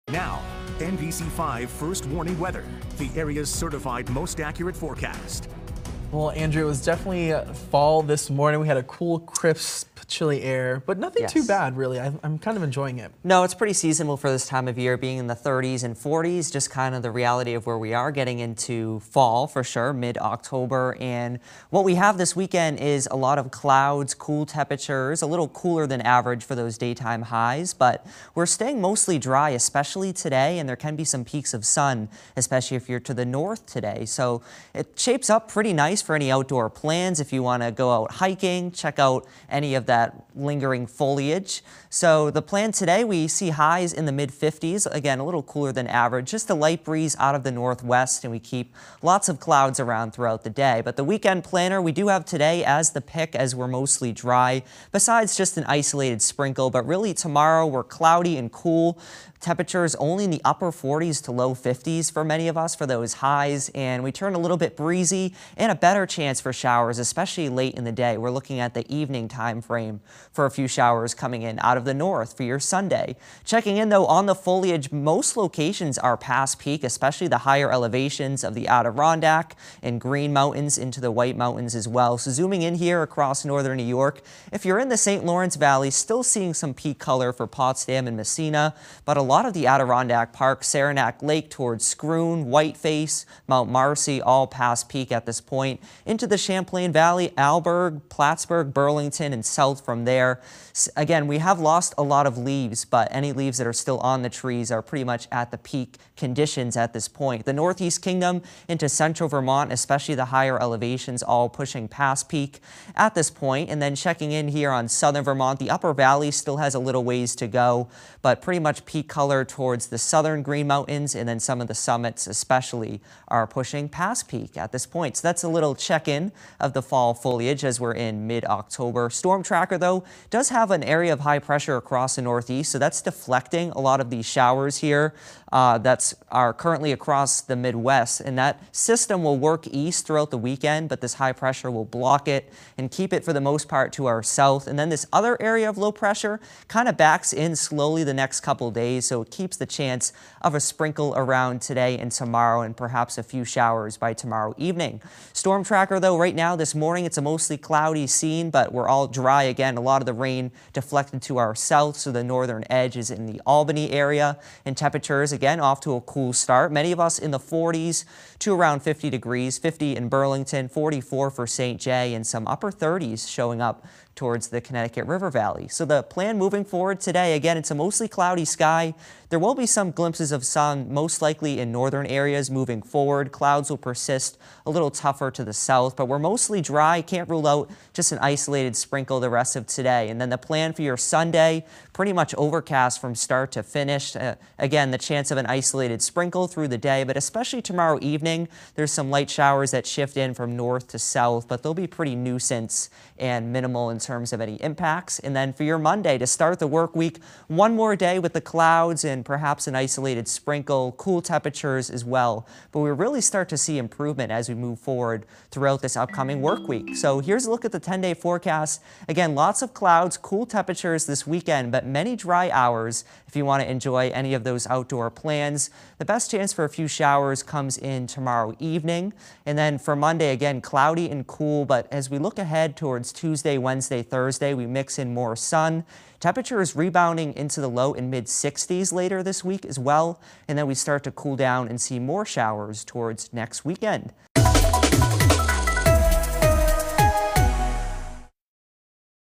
Video: Cool, mostly dry weekend (10-14-23)
Andrew times out a few showers for Sunday and looks ahead to warmer temperatures for next week.